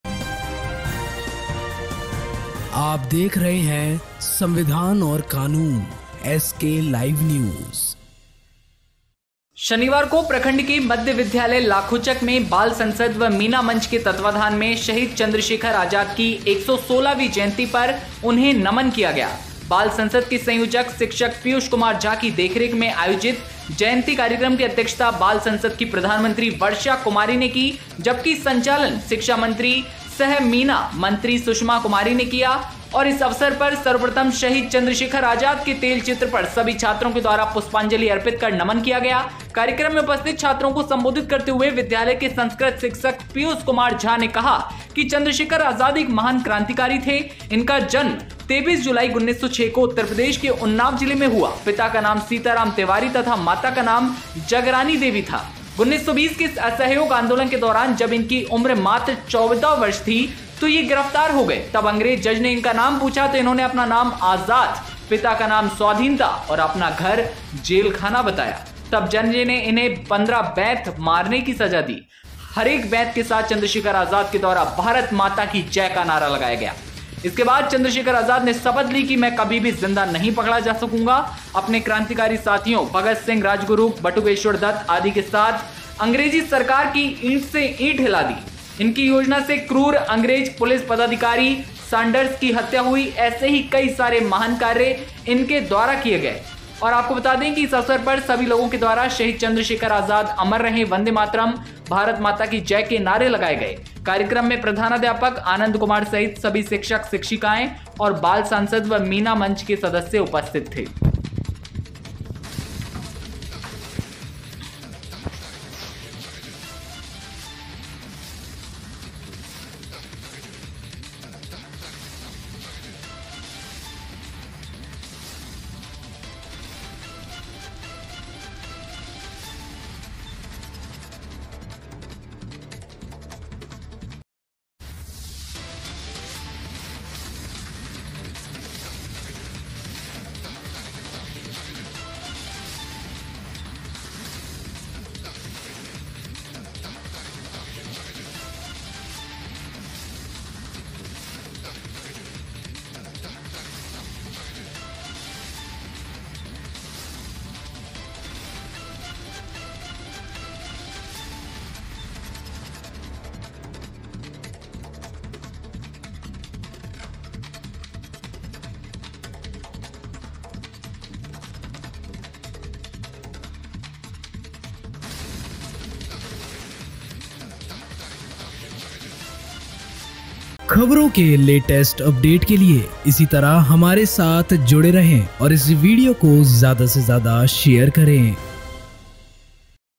मध्य विद्यालय लाखोचक में चन्द्रशेखर आजाद कि मनी 116 वीं जयन्ती
शनिवार को प्रखंड के मध्य विद्यालय लाखोचक में बाल संसद व मीना मंच के तत्वावधान में शहीद चंद्रशेखर आजाद की 116वीं जयंती पर उन्हें नमन किया गया। बाल संसद के संयोजक शिक्षक पीयूष कुमार झा की देखरेख में आयोजित जयंती कार्यक्रम की अध्यक्षता बाल संसद की प्रधानमंत्री वर्षा कुमारी ने की जबकि संचालन शिक्षा मंत्री सह मीना मंत्री सुषमा कुमारी ने किया। इस अवसर पर सर्वप्रथम शहीद चंद्रशेखर आजाद के तैल चित्र पर सभी छात्रों के द्वारा पुष्पांजलि अर्पित कर नमन किया गया। कार्यक्रम में उपस्थित छात्रों को संबोधित करते हुए विद्यालय के संस्कृत शिक्षक पीयूष कुमार झा ने कहा कि चंद्रशेखर आजाद एक महान क्रांतिकारी थे ।
इनका जन्म 23 जुलाई 1906 को उत्तर प्रदेश के उन्नाव जिले में हुआ।  पिता का नाम सीताराम तिवारी तथा माता का नाम जगरानी देवी था।
 1920 के असहयोग आंदोलन के दौरान जब इनकी उम्र मात्र 14 वर्ष थी ये गिरफ्तार हो गये। जब अंग्रेज जज ने इनका नाम पूछा तो इन्होंने अपना नाम आजाद, पिता का नाम स्वाधीनता तथा अपना घर जेल खाना बताया। तब जज ने इन्हें 15 बेंत मारने की सजा दी। हरेक बेंत के साथ चंद्रशेखर आजाद के द्वारा भारत माता की जय का नारा लगाया गया। इसके बाद चंद्रशेखर आजाद ने शपथ ली की मैं कभी भी जिंदा नहीं पकड़ा जा सकूंगा। अपने क्रांतिकारी साथियों भगत सिंह, राजगुरु, बटुकेश्वर दत्त आदि के साथ अंग्रेजी सरकार की ईंट से ईंट हिला दी । इनकी योजना से क्रूर अंग्रेज पुलिस पदाधिकारी सांडर्स की हत्या तथा केंद्रीय असेम्बली में बम फेंके जाने की घटनाएं हुई। 27 फरवरी 1931को लखनऊ के अल्फ्रेड पार्क में इनकी मृत्यु अंग्रेजों से मुठभेड़ के बाद अंतिम गोली स्वयं मारकर इस वीर सपूत ने अपने प्राणों की आहुति दे दी। इस अवसर पर सभी लोगों के द्वारा शहीद चंद्रशेखर आजाद अमर रहें, वंदे मातरम,भारत माता की जय नारे लगाए गए। कार्यक्रम में प्रधानाध्यापक आनंद कुमार सहित सभी शिक्षक शिक्षिकाएं तथा बाल संसद व मीना मंच के सदस्य उपस्थित थे।